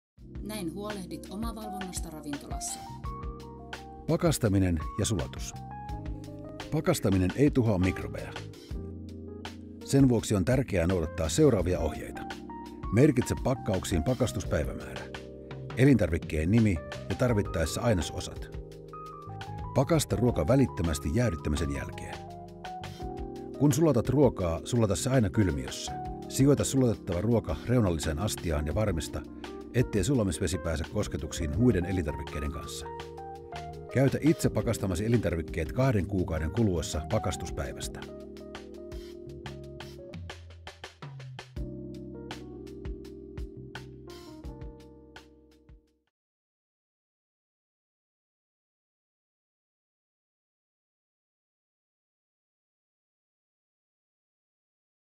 Näin huolehdit omavalvonnasta: Pakastaminen ja sulatus
Pakastaminen ei tuhoa mikrobeja. Sen vuoksi on tärkeää esimerkiksi pakastaa ruoka heti jäähdyttämisen jälkeen.